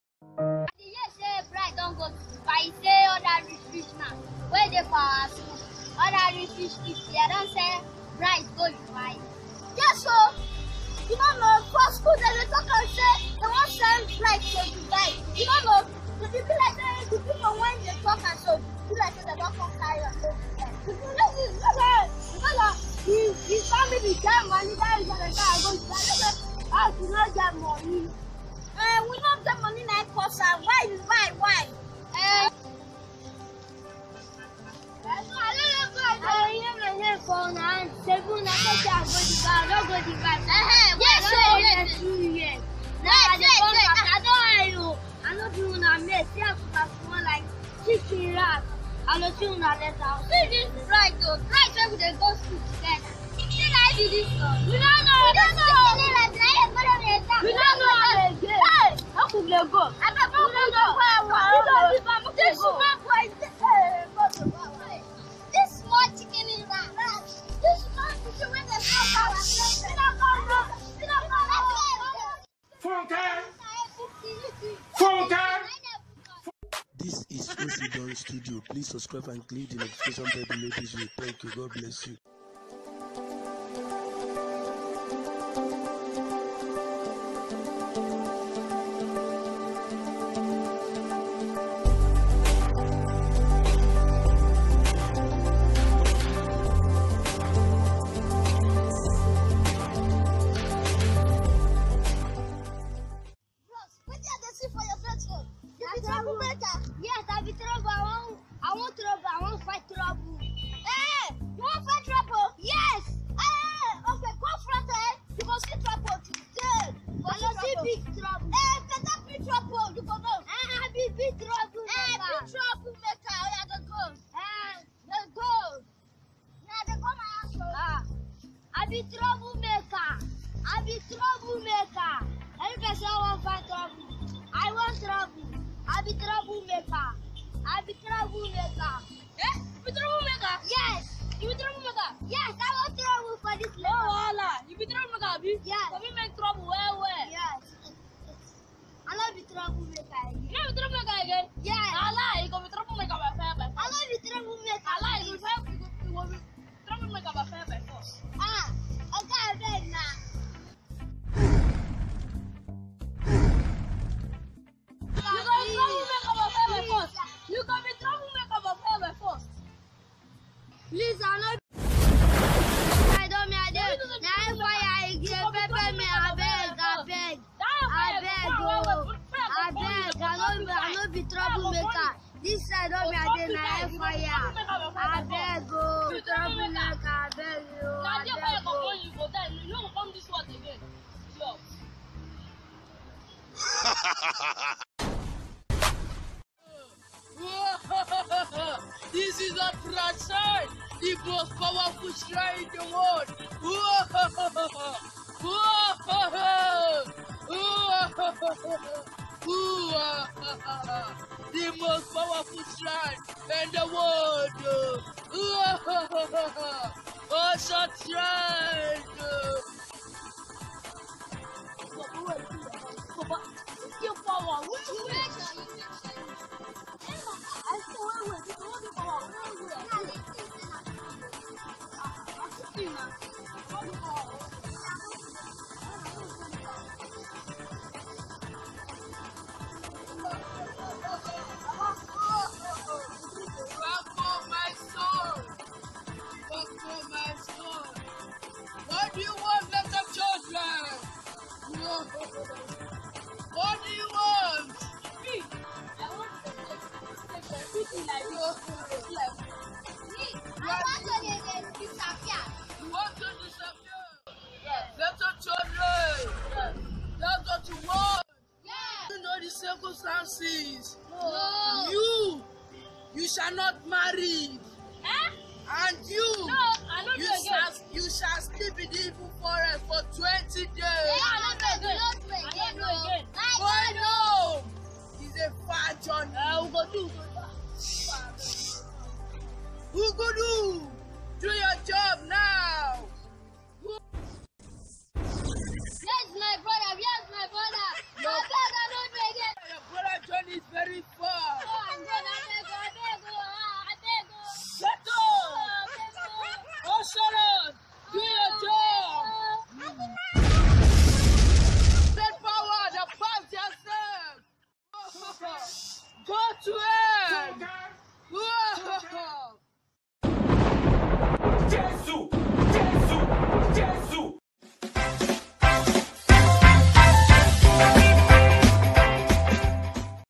SHRINES OCY DUN MICHAEL TV
Thisvideo was brought to you by Ocy Dun from the very studio of Michael, watch watch this movie called shrines to laugh as you will see these wonderful children performing in this series, please subscribe to our channel and share this video, thanks